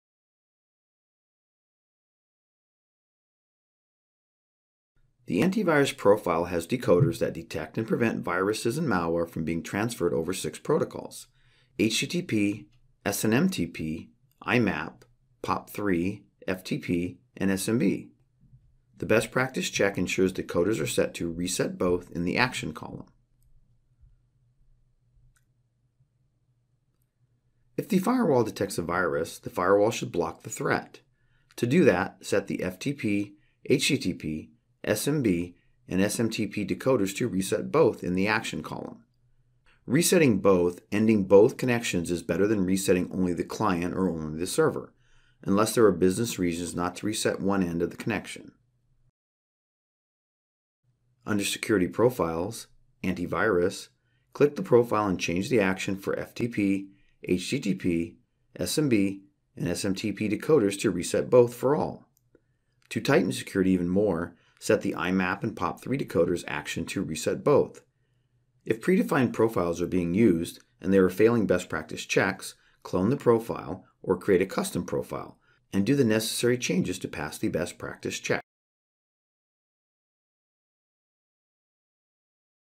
AntiVirus Decoder Actions - Interpreting BPA Checks - Objects
This video covers the importance of antivirus decoder actions best practice check and what happens when the firewall detects a virus.

This Antivirus profile has decoders that detect and prevent viruses and malware from being transferred over six protocols: HTTP, SMTP, IMAP, POP3, FTP, and SMB. The Decoder Actions best practice check ensures the decoders are set to Reset-Both in the Action Column.